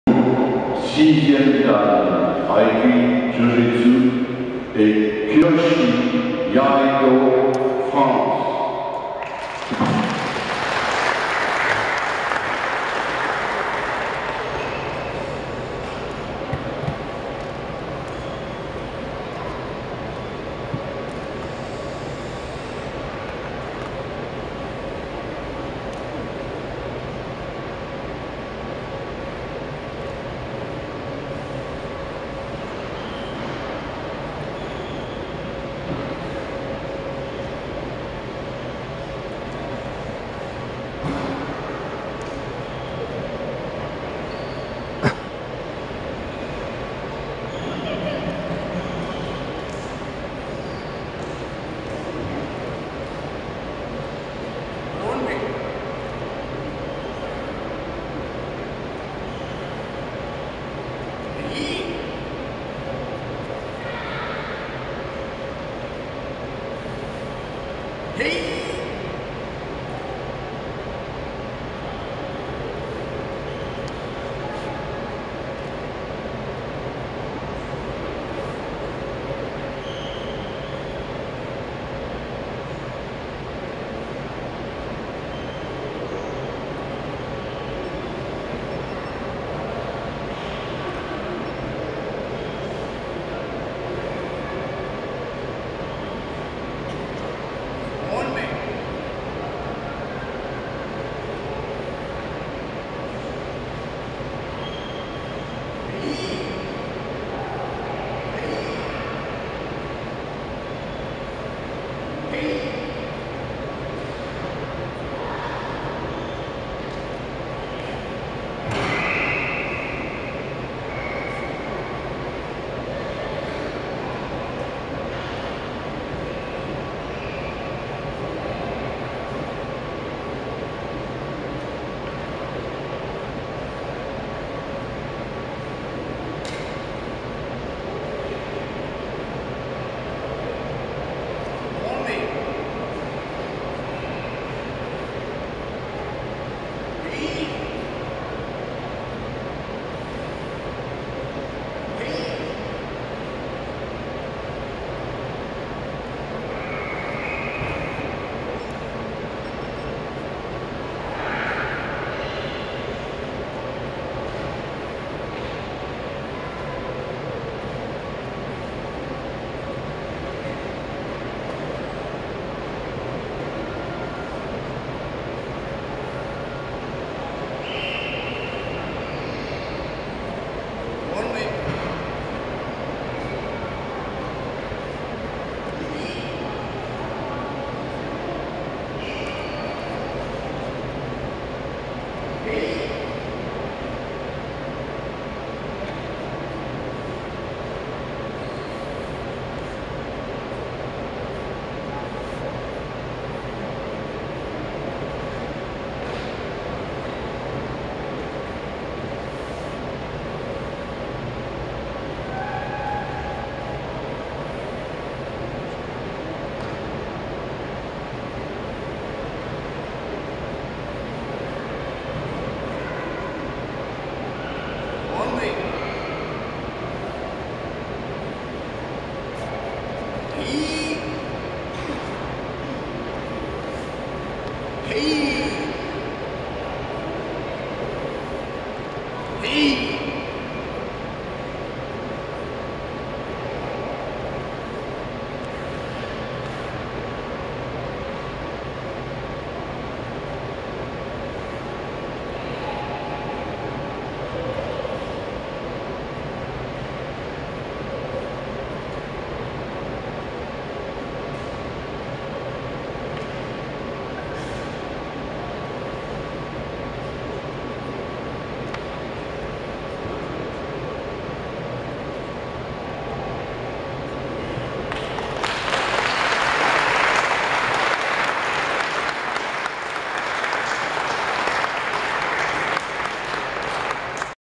Taikai 2019 - Jacques POU (6 dan, France) Demonstration
The demonstration presented by Jacques POU (6 dan, France) on "First European Taikai Aiki-Jujutsu Takeda-Ryu MAROTO-HA", Alexandria, Romania, 14-16 June 2019